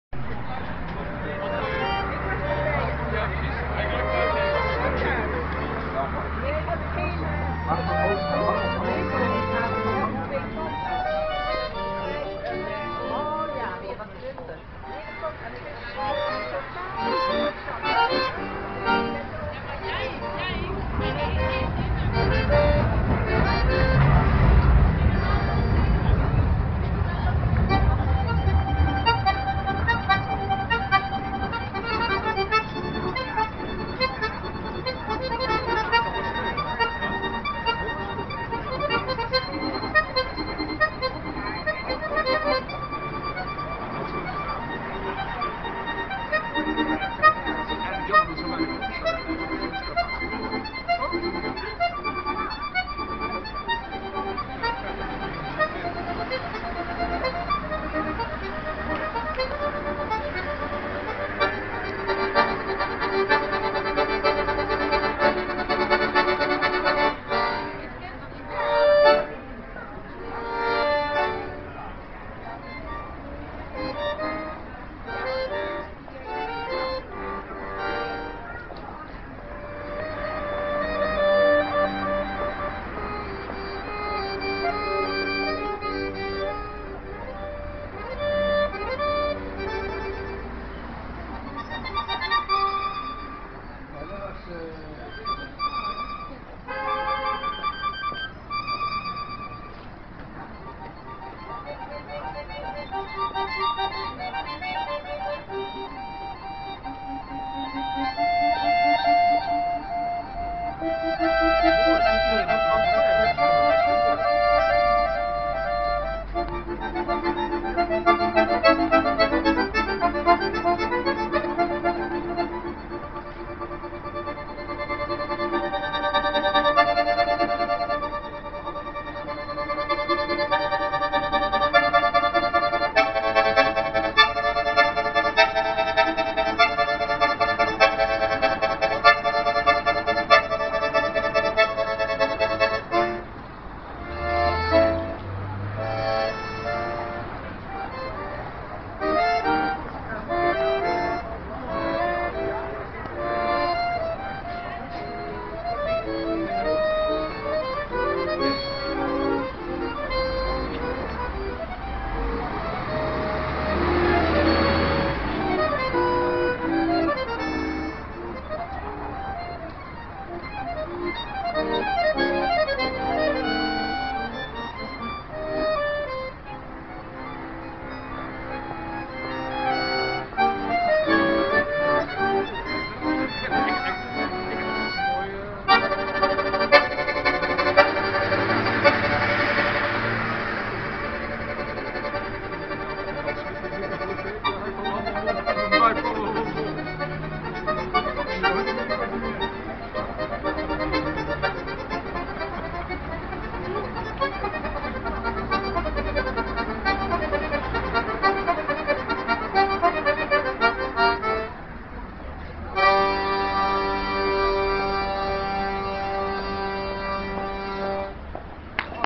Accordeon Vivaldi Straatmuzikant
Op 4 november 2007 bij het Concertgebouw te Amsterdam